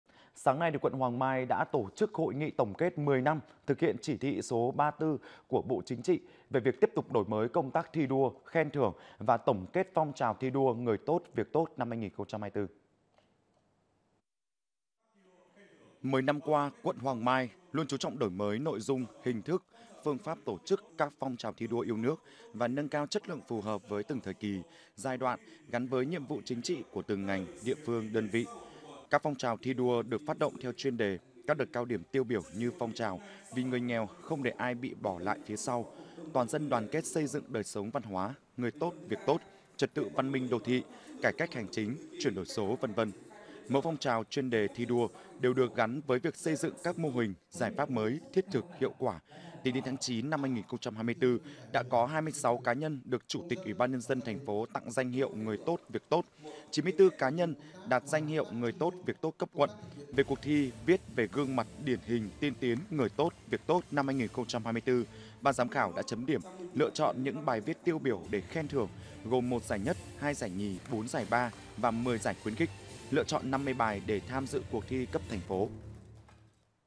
Hoàng Mai có 26 cá nhân được công nhận "Người tốt việc tốt" cấp thành phố | Tin tức
Sáng nay, quận Hoàng Mai đã tổ chức Hội nghị tổng kết 10 năm thực hiện Chỉ thị số 34 của Bộ Chính trị về tiếp tục đổi mới công tác thi đua, khen thưởng và tổng kết phong trào thi đua “Người tốt, việc tốt” năm 2024.